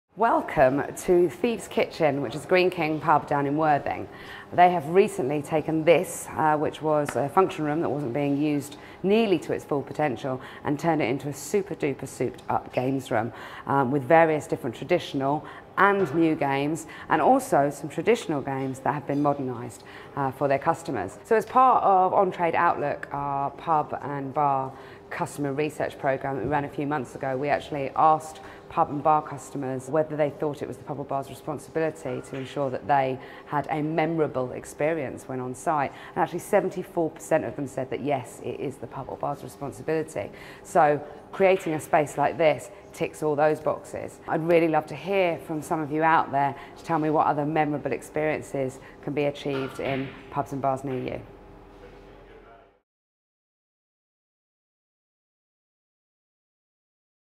KAM Vlog: Pub Experiences - Thieves' Kitchen, Worthing
Katy visits Thieves' Kitchen, a Greene King pub in Worthing, to check out their amazing new games room and the experiences they offer their customers.